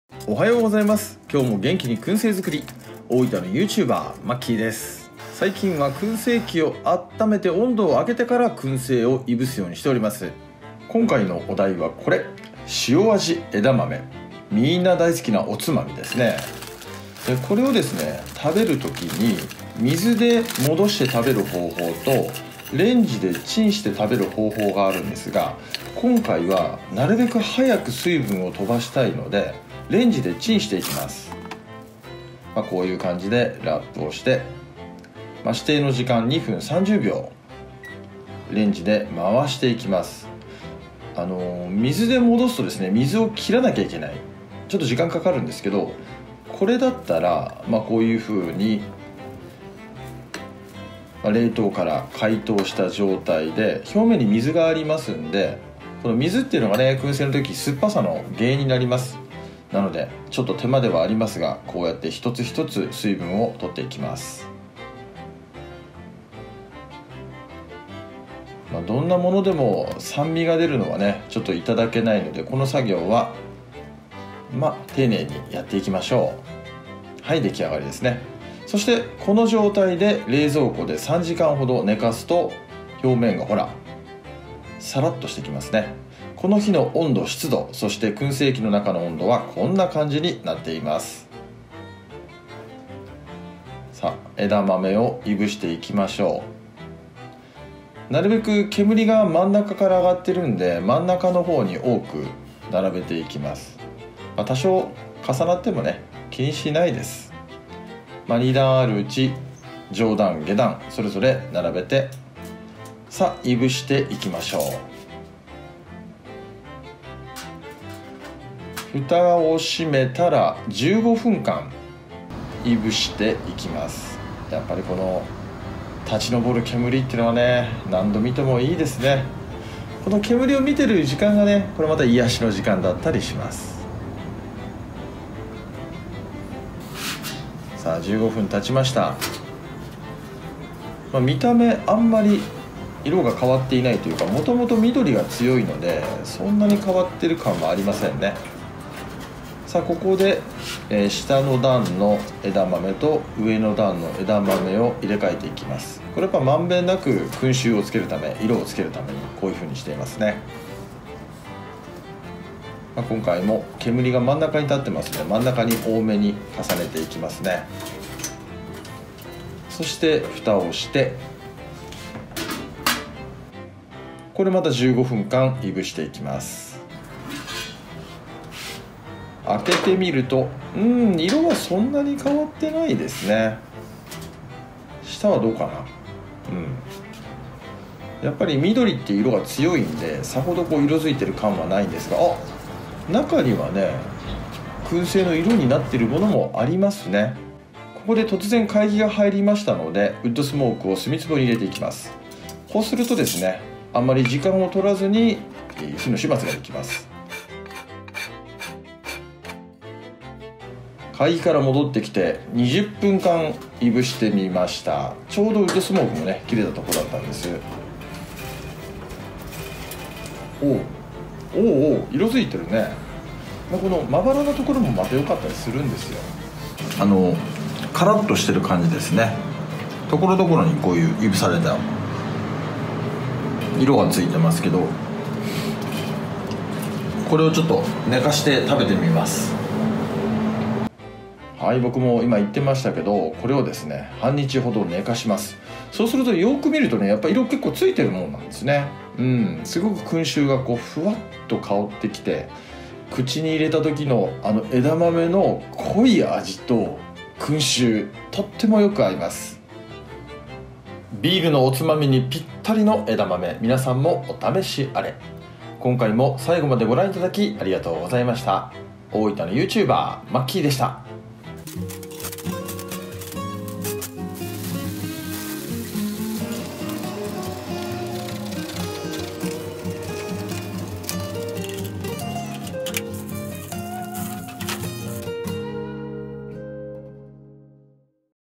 【燻製】ほぼ週な燻製日記 枝豆の燻製は美味しいのか！？【キャンプ】
大分のYouTuber、MAKKYです！

カスタムした燻製器、SOTOお手軽香房ST-124で燻製作り。

おつまみの人気アイテム、枝豆の燻製は美味しいのか！？

房#枝豆#冷凍食品#いぶり暮らし